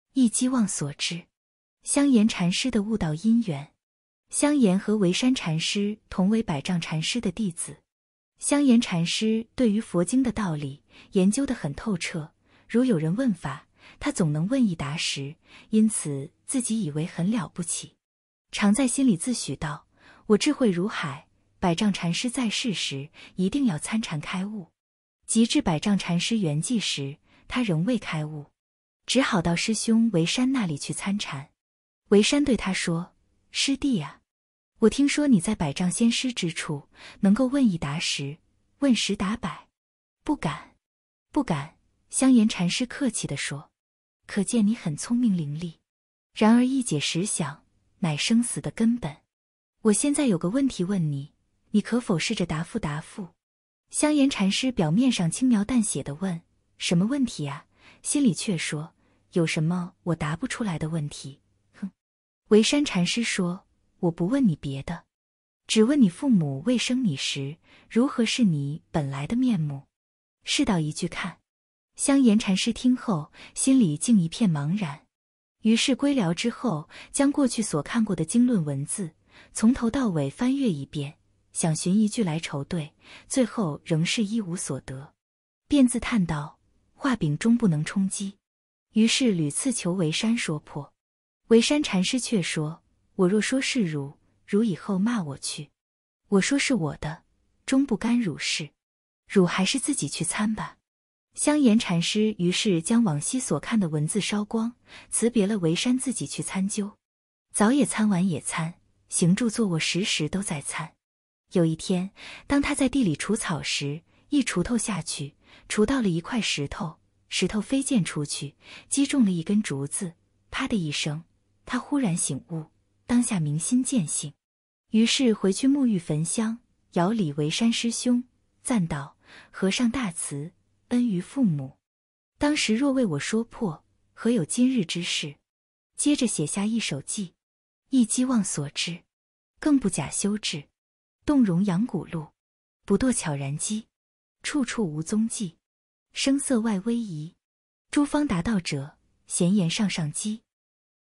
【一擊忘所知】 香嚴禪師的悟道因緣
香嚴禪師和潙山禪師同為百丈禪師的弟子。香嚴禪師對於佛經的道理，研究得很透徹，如有人問法，他總能問一答十，因此自己以為很了不起。常在心裡自許道：「我智慧如海，百丈禪師在世時，一定要參禪開悟！」

及至百丈禪師圓寂時，他仍未開悟。只好到師兄潙山那裡去參禪。潙山對他說：「師弟啊！我聽說你在百丈先師之處，能夠問一答十，問十答百。」「不敢！不敢！」香嚴禪師客氣地說。

「可見你很聰明伶俐！然而意解識想，乃生死的根本。我現在有個問題問你，你可否試著答覆答覆？」

香嚴禪師表面上輕描淡寫地問：「什麼問題呀？」心裡卻說：「有什麼我答不出來的問題，哼！」

潙山禪師說：「我不問你別的，只問你父母未生你時，如何是你本來的面目？試道一句看。」

香嚴禪師聽後，心裡竟一片茫然。於是歸寮之後，將過去所看過的經論文字，從頭到尾翻閱一遍，想尋一句來酬對，最後仍是一無所得。便自嘆道：「畫餅終不能充饑。」

於是屢次求潙山說破。潙山禪師卻說：「我若說示汝，汝以後罵我去。我說是我的，終不干汝事。汝還是自己去參吧！」

香嚴禪師於是將往昔所看的文字燒光，辭別了潙山自己去參究。早也參晚也參，行住坐臥時時都在參。

有一天，當他在地裡除草時，一鋤頭下去，鋤到了一塊石頭，石頭飛濺出去，擊中了一根竹子，「啪」的一聲，他忽然醒悟，當下明心見性！

於是回去沐浴焚香，遙禮潙山師兄，讚道：「和尚大慈，恩逾父母，當時若為我說破，何有今日之事？」
接著寫下一首偈：
「一擊忘所知，更不假修治；動容揚古路，不墮悄然機。處處無蹤跡，聲色外威儀，諸方達道者，咸言上上機。」